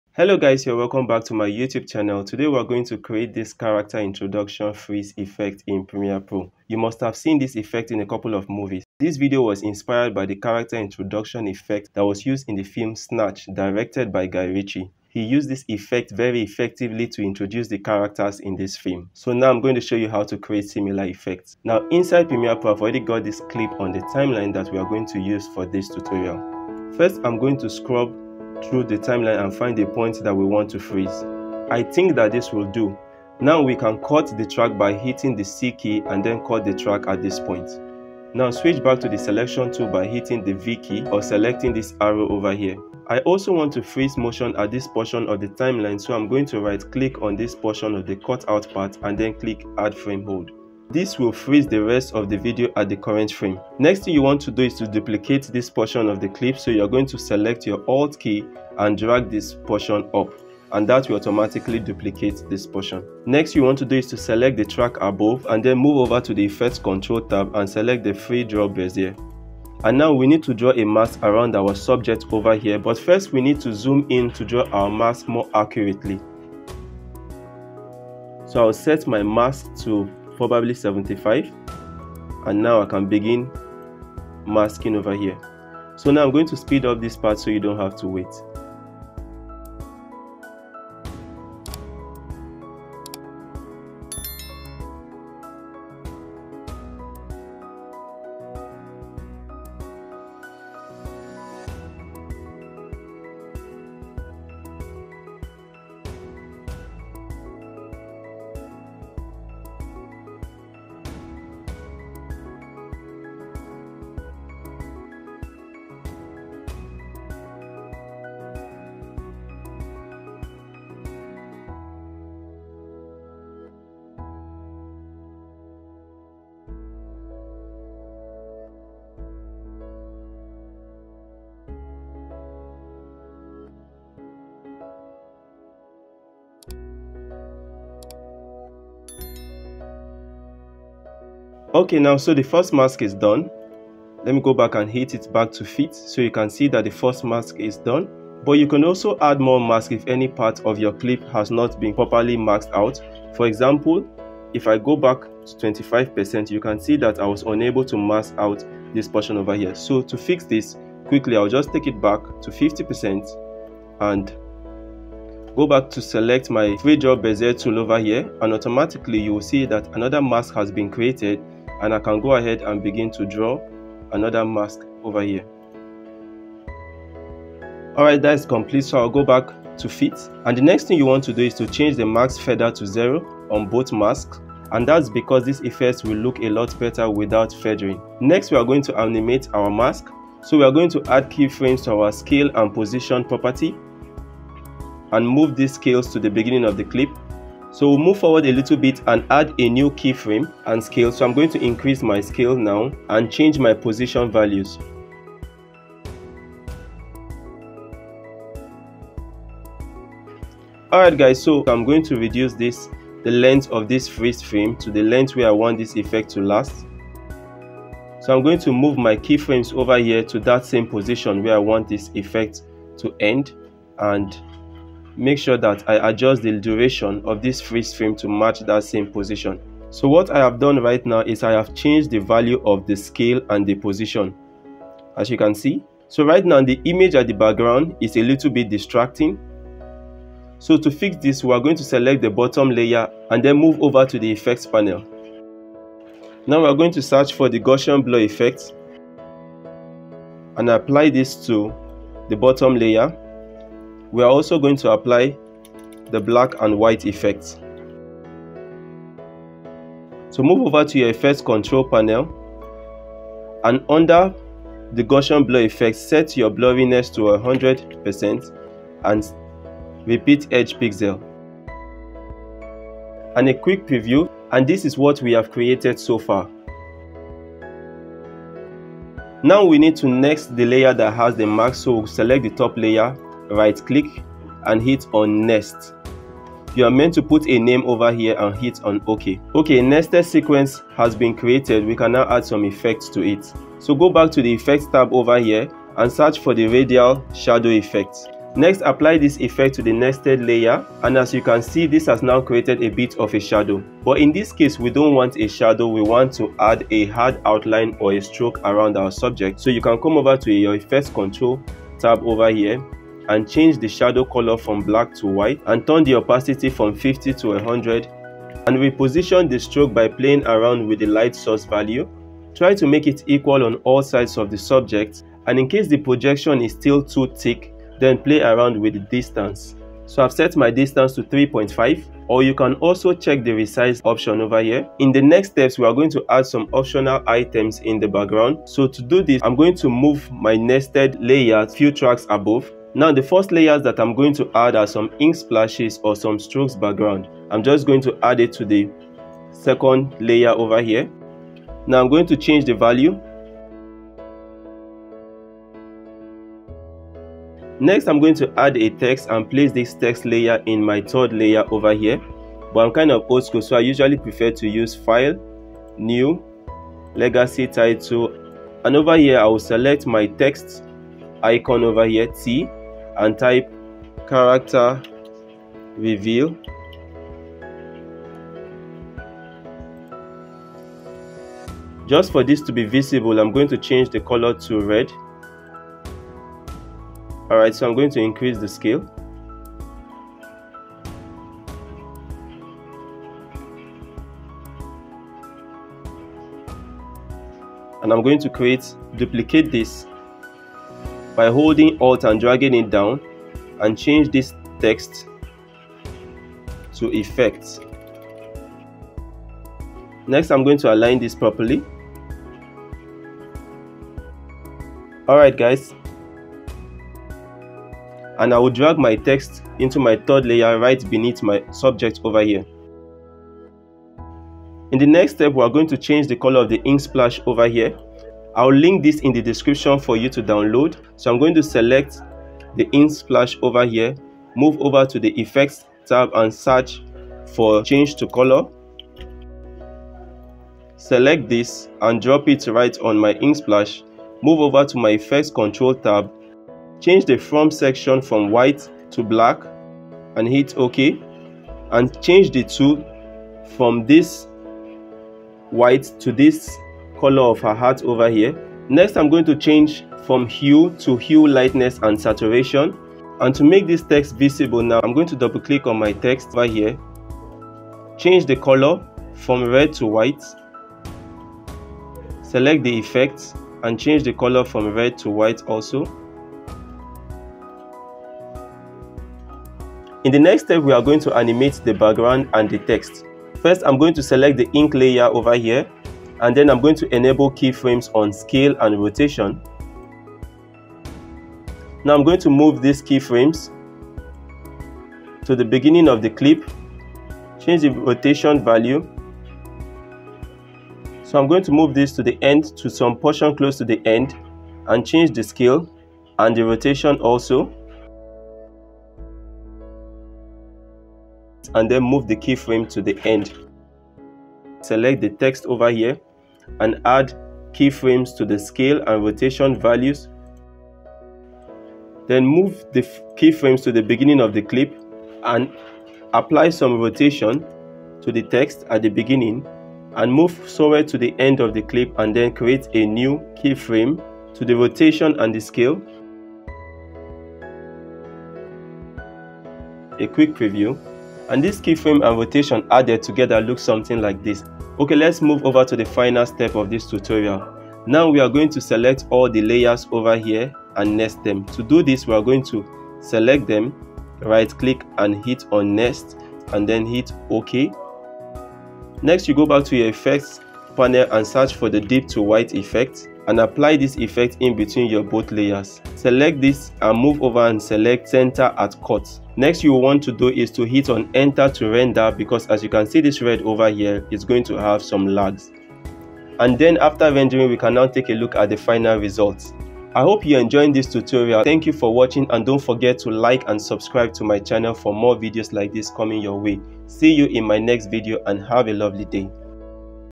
How To Create Character Introduction Freeze Effect in Premiere Pro cc ( Tutorial)
Hello Creative People !

Welcome Back To Another Brand New premiere pro cc Tutorial By ivisualpro

In This Tutorial Video I'm Going To Show You How To Create The popular Character Introduction freeze Effect Using Adobe Premiere Pro Cc

To create this effect, We Will First Freeze The Frame Then duplicate the freezed frame one track above and Make A Mask Around The Subject. Then Make The Background Blur Out and also add the black & white effect to the background So That Our Character Pop Out From The Frame.

So Guys We Have A Great Video For Today So Lets Get Started And Have Some Fun With  Premiere Pro Creating this Character Introduction Freeze Effect.

00:00 - Intro
00:07 - Tutorial
09:45 - outro